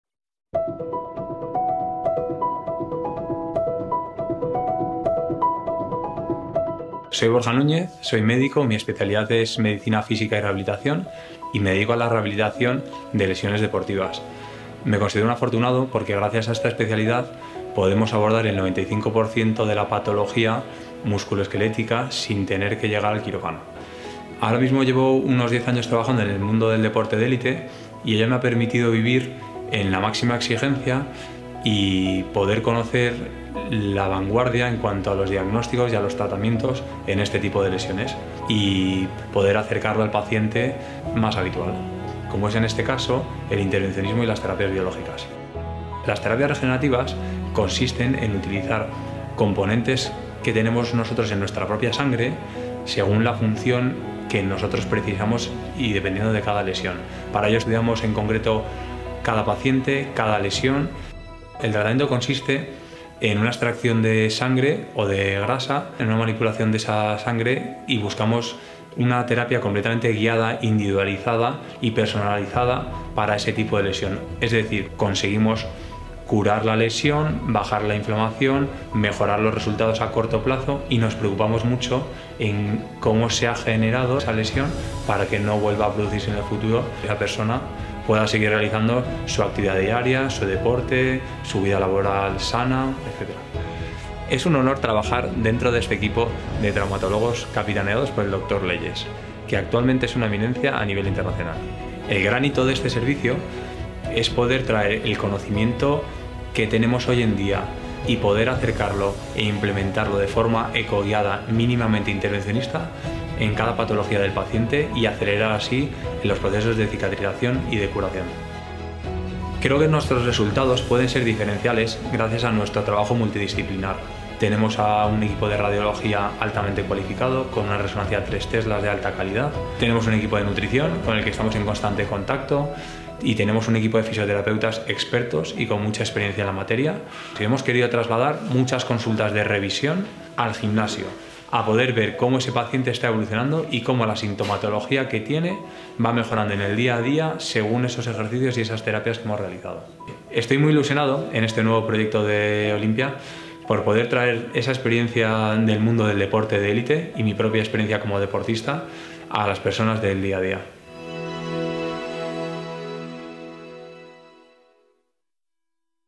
Unidad de Intervencionismo y Terapias Biológicas y Regenerativas de Olympia
En la Unidad de Intervencionismo y Terapias Biológicas y Regenerativas de Olympia, el Dr. Borja Núñez se encarga de proporcionar al paciente tratamientos alternativos para curar sus lesiones sin tener que llegar al quirófano. Gracias a las infiltraciones de plasma rico en plaquetas (PRP) con tecnología de última generación, somos capaces de personalizar cada terapia para asegura así una pronta y efectiva recuperación de las dolencias de los pacientes.